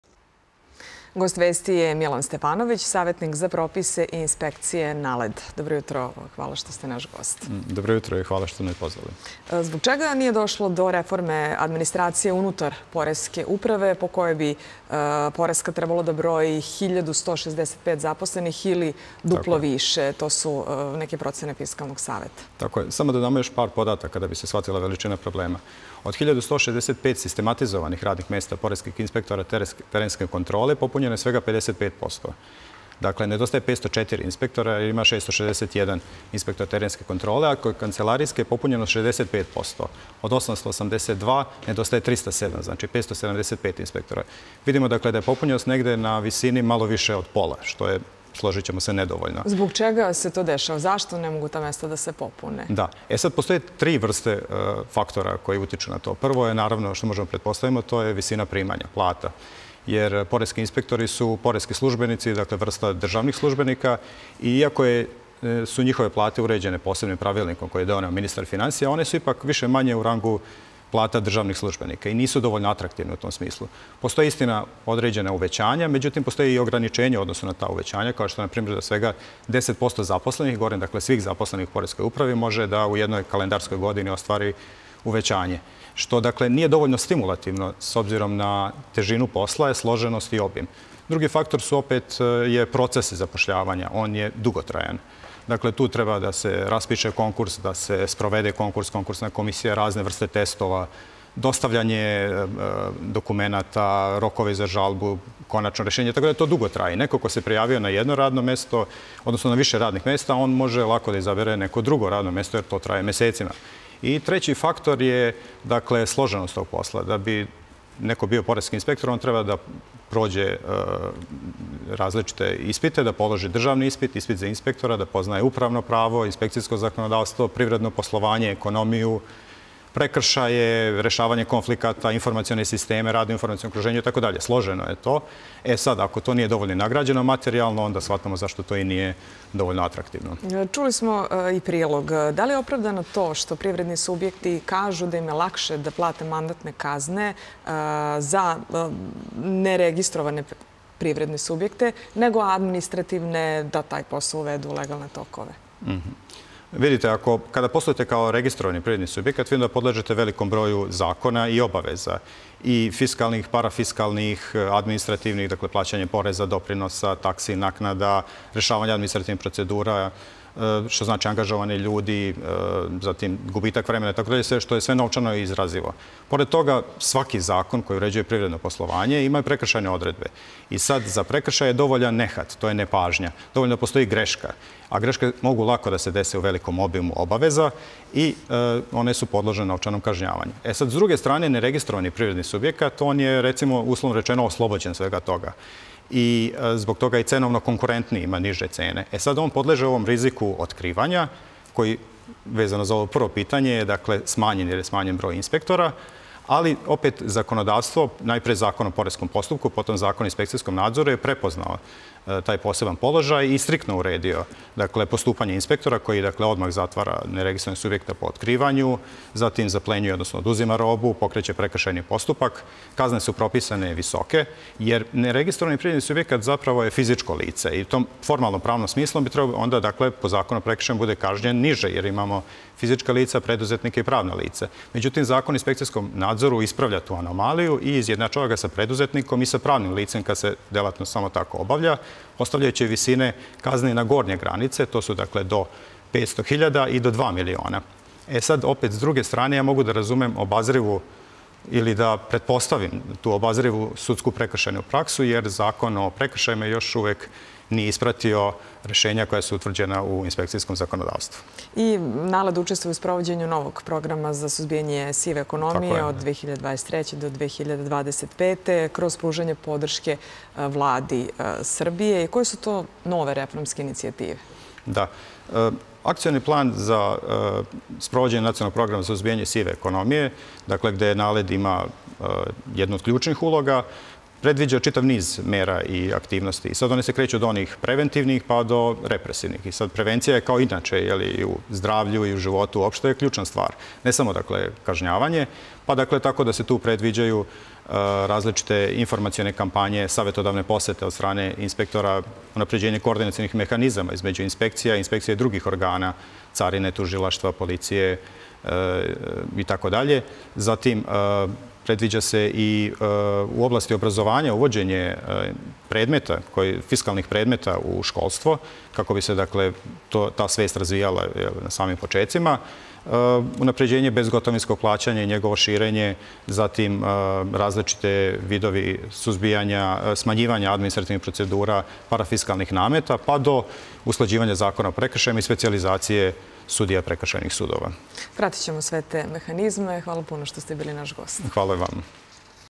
[Euronews] Zašto Poreska uprava još uvek nema dovoljan broj poreskih inspektora?
Savetnik za propise i inspekcije u NALED-u Milan Stefanović govorio je za Euronews o dugogodišnjem problemu Poreske uprave da popuni predviđenih  1165 sistematizovanih radnih mesta za poreske inspektore. Do danas, uposleno je svega 55% mesta od predviđenog broja.